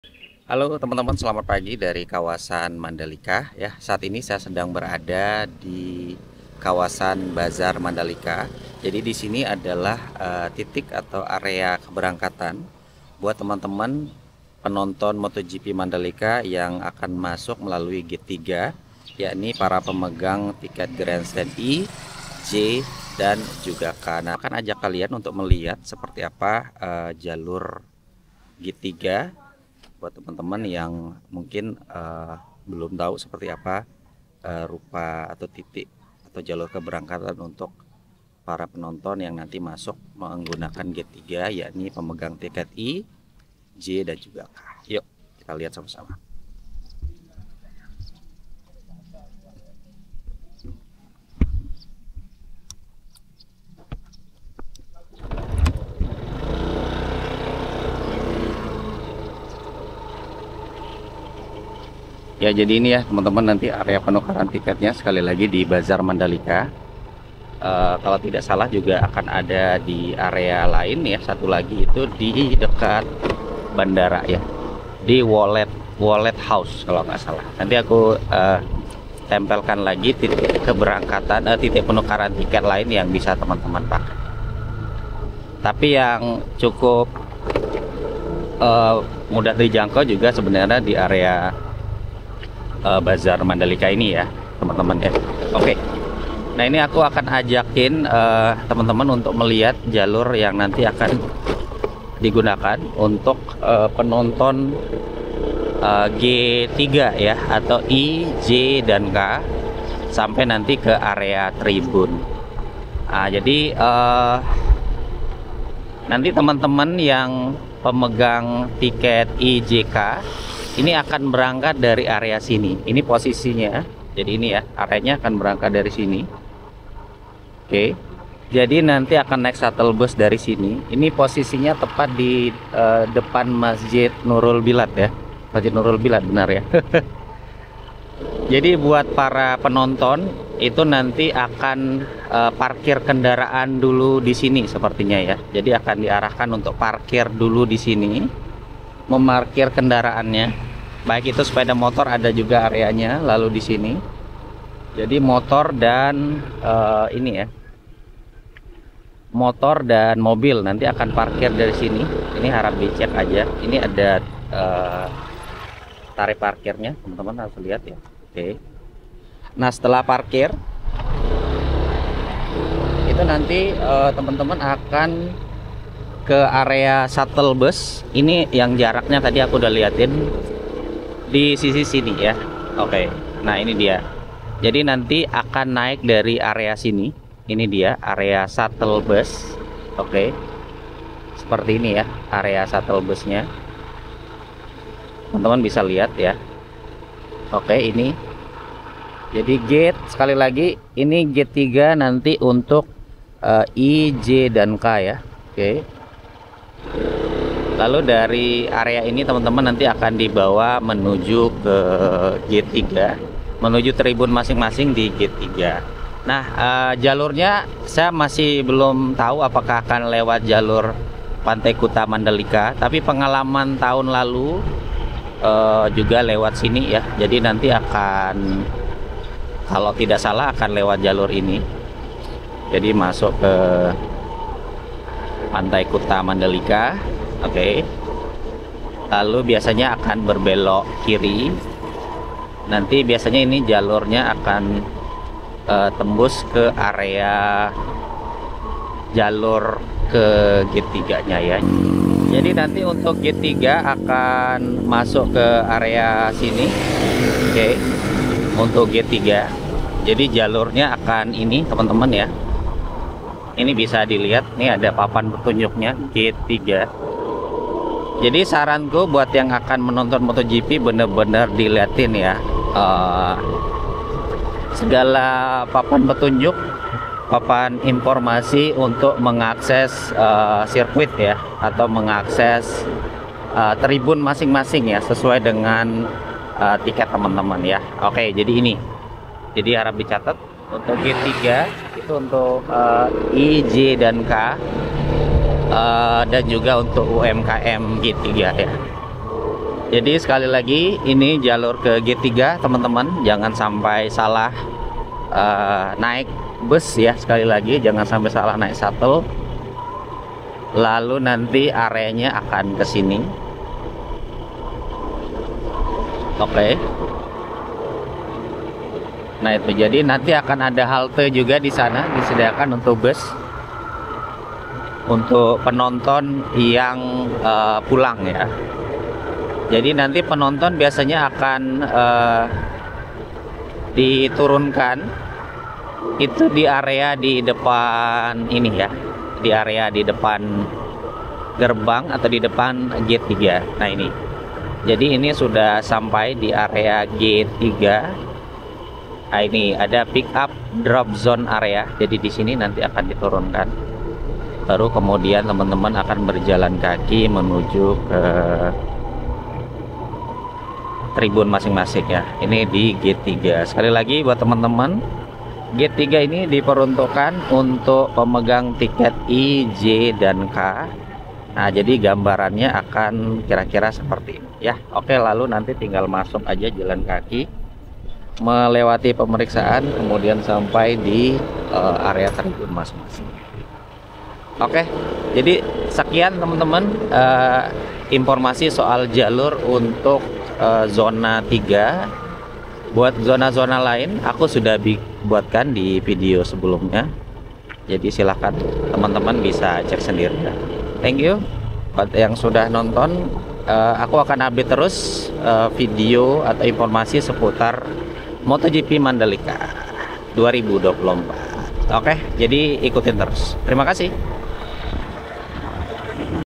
Jangan Salah, Ini Jalur Pemegang Tiket I, J, dan K MotoGP Mandalika
Begini suasana jalur Gate 3 penonton MotoGP pemegang tiket I, J, dan K MotoGP Mandalika.